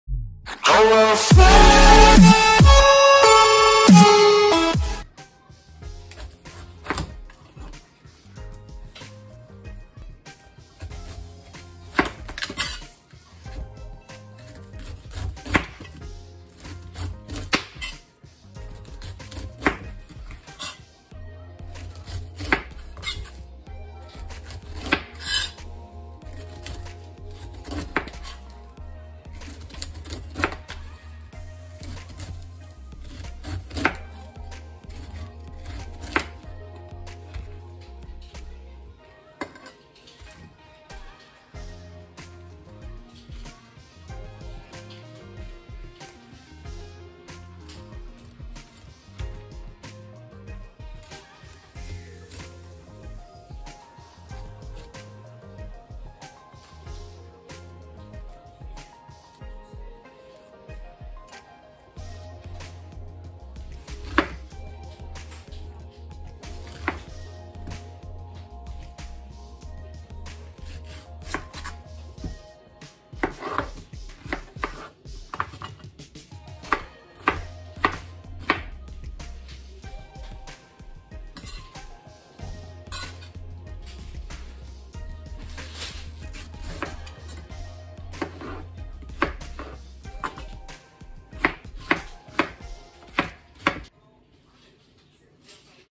Rosalyn Mizuno HOW TO CUT PINEAPPLE QUICK AND EASY WAY
Pineapple is very healthy.it is loaded with nutrients,contains disease fighting antioxidants.May help reduce cancer.May boost Immunity and suppress inflamation etc.